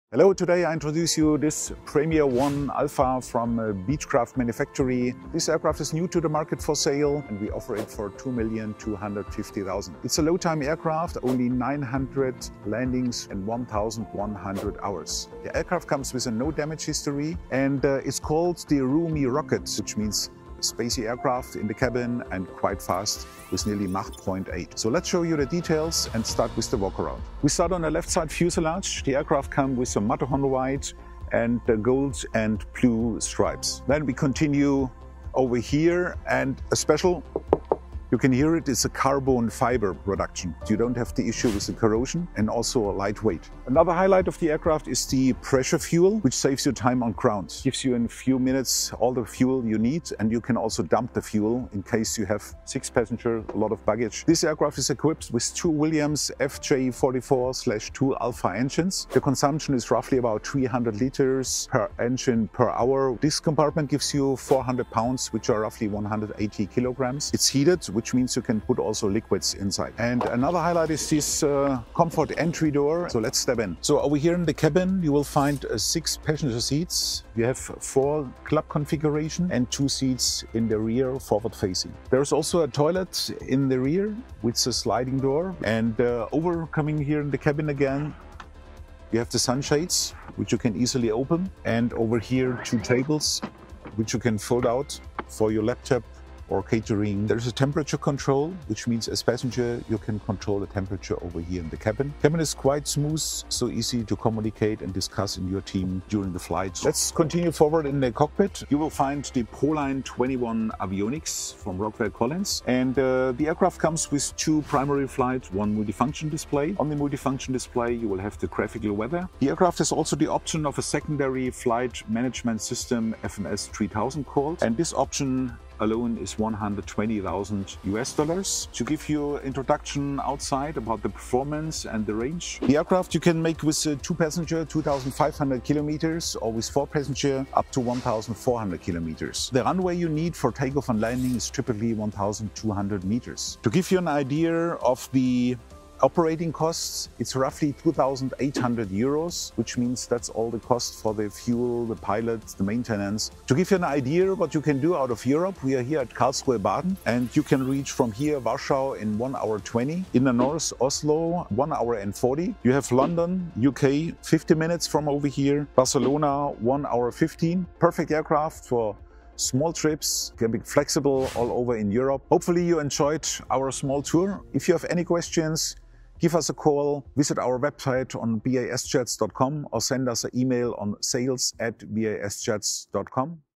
2010 Beechcraft Premier 1A
2010 Beechcraft Premier 1A for sale by BAS

TTAF 1.100 Low Time
Landings 900
Dual FMS 3000
Proline 21 Avionics Dual FMS 3000

For the latest aircraft for sale, intelligence, aviation services, and more,